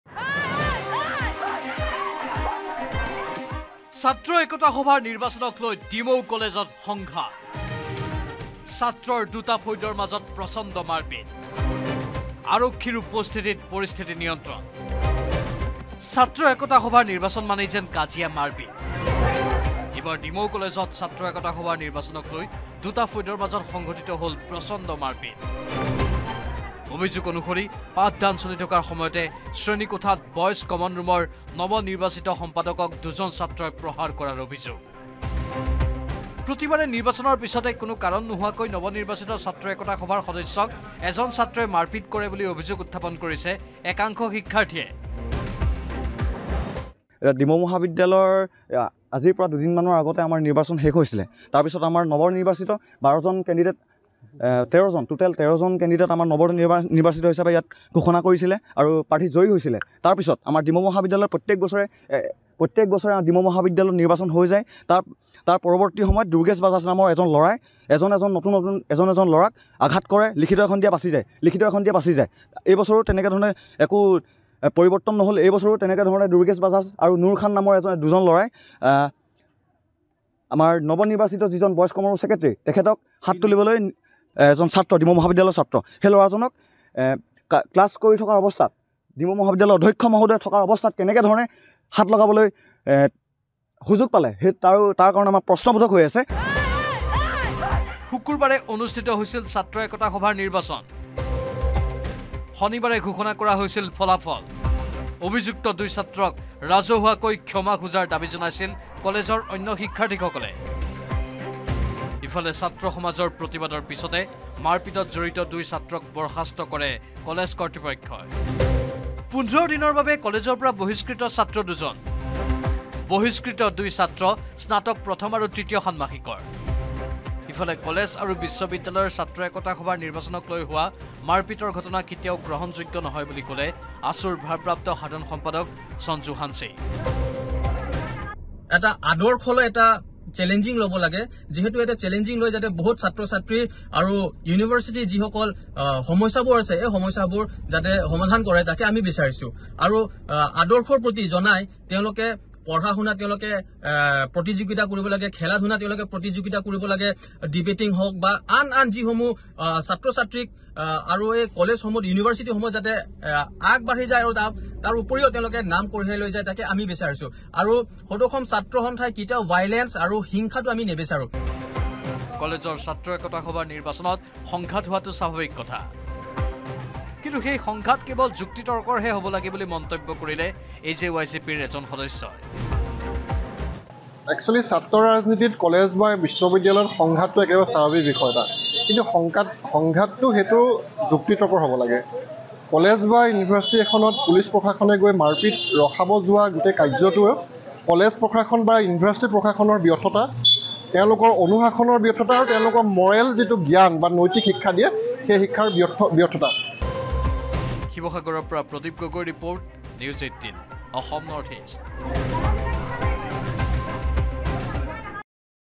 Demow College Fight | ছাত্ৰ একতা সভাৰ নিৰ্বাচনক লৈ ডিমৌ কলেজত সংঘাত | N18V
Demow College Fight |  ছাত্ৰ একতা সভাৰ নিৰ্বাচনক লৈ ডিমৌ কলেজত সংঘাত
ছাত্ৰৰ দুটা ফৈদৰ মাজত প্ৰচণ্ড মাৰপিট
আৰক্ষীৰ উপস্থিতিত পৰিস্থিতি নিয়ন্ত্ৰণ

লগতে চাওঁক দিনটোৰ বহু গুৰুত্বপূৰ্ণ খবৰ |